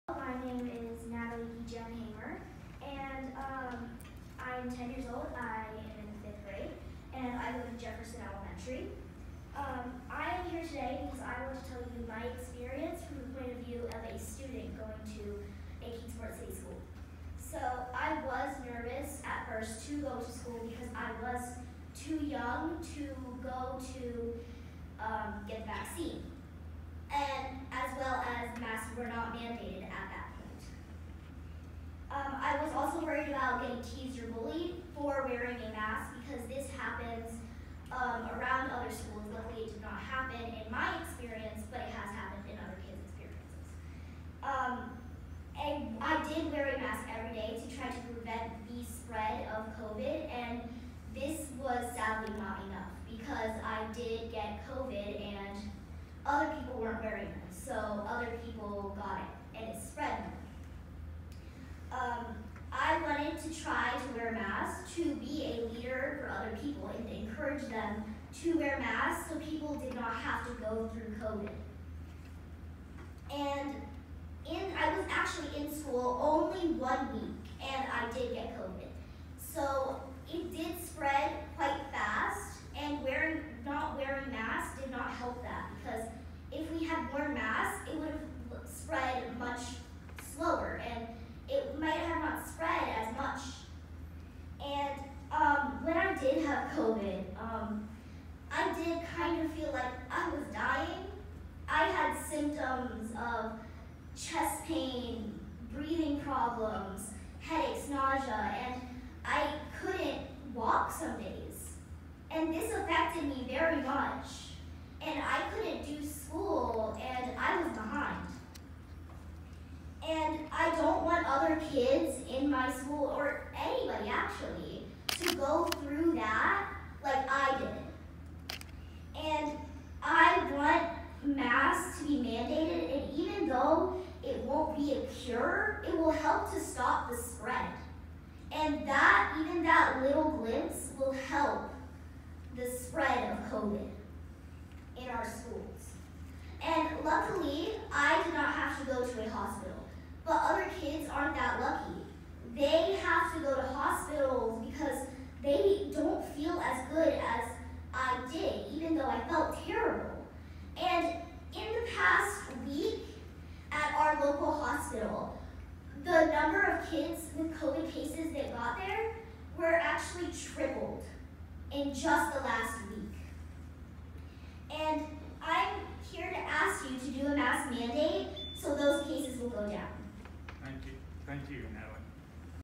Natalie Guillen-Hamer
Natalie Guillen-Hamer, a Jefferson Elementary Schools fifth grader, speaks in favor of a mask mandate during Friday's called Kingsport school board meeting. The 10-year-old got COVID, which her mother said she believes came from school.